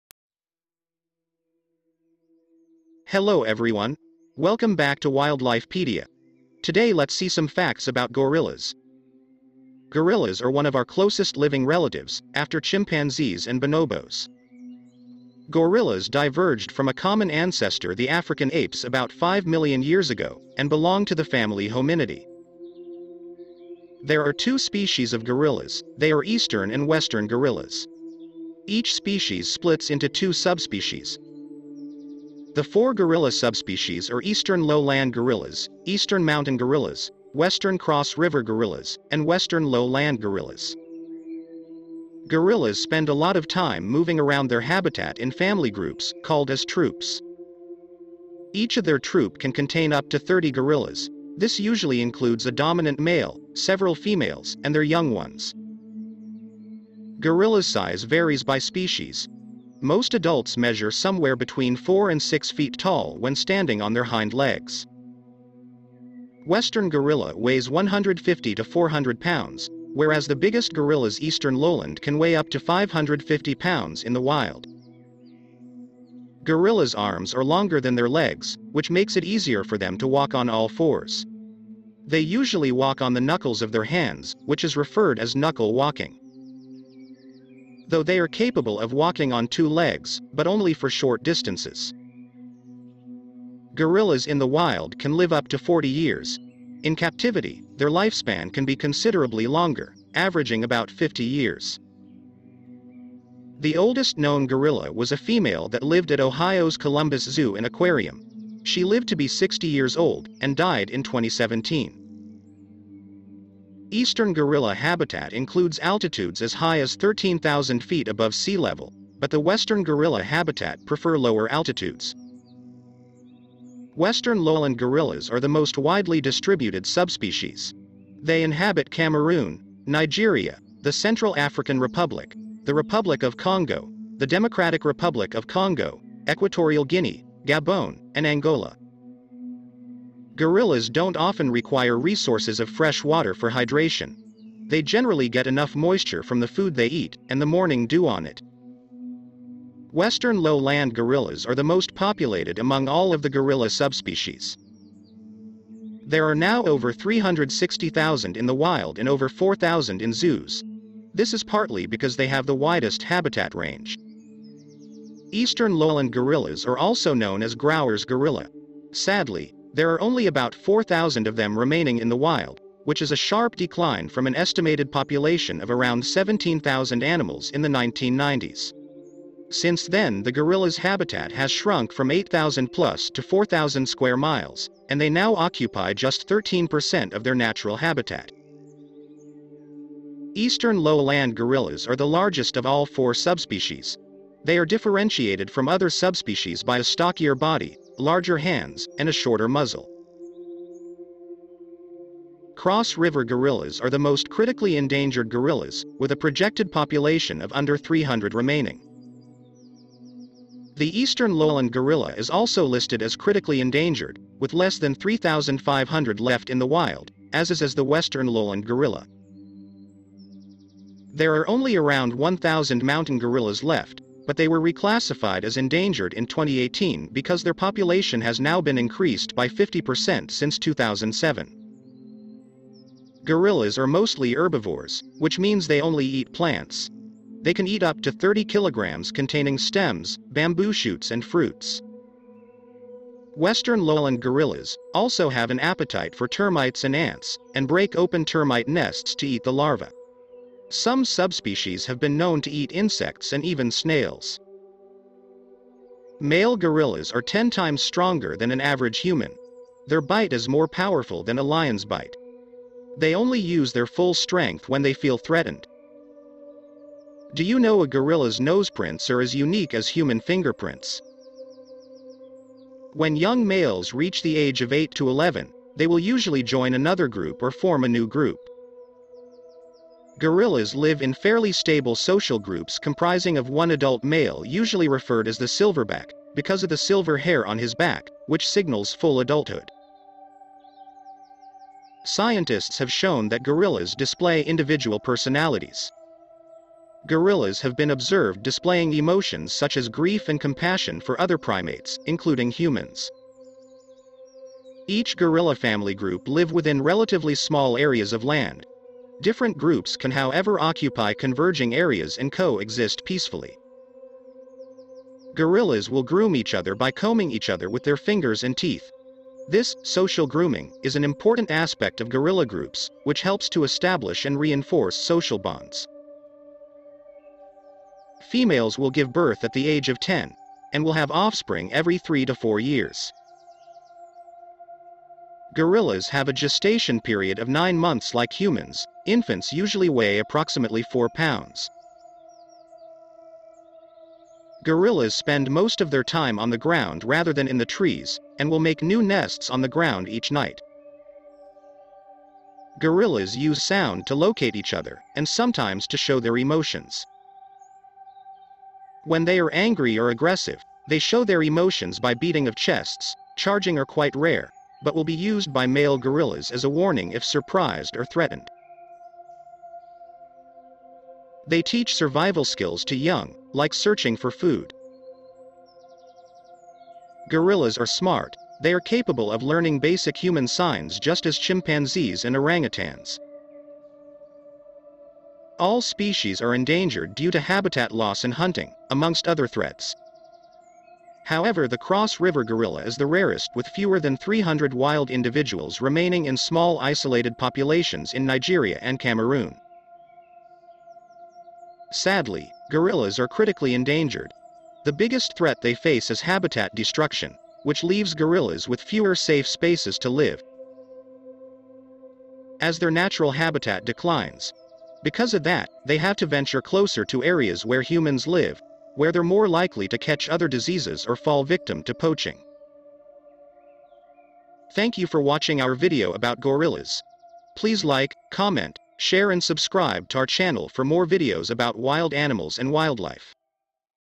Gorilla's: Surprising Facts About Gorillas | Wild Animal Species Expedition
There are two species of gorillas they are eastern and western gorillas.
Each species splits into two subspecies. The four gorilla subspecies are eastern lowland gorillas, eastern mountain gorillas, western cross river gorillas, and western lowland gorillas.
Gorillas spend a lot of time moving around their habitat in family groups called as troops..
Each of their troop can contain up to 30 gorillas This usually includes a dominant male, several females, and their young ones. Though gorilla size varies by species, most adults measure somewhere between four and six feet tall when standing on their hind legs.
Gorilla weight also varies by species: The smaller Western Gorilla weighs 150 to 400 pounds, whereas the biggest gorillas (Eastern Lowland) can weigh up to 550 pounds in the wild.
Gorillas arms are longer than their legs, which makes it easier for them to walk on all fours.
They usually walk on the knuckles of their hands, which is appropriately referred to as knuckle walking. Though they are capable of walking on two legs, they can only do so for short distances.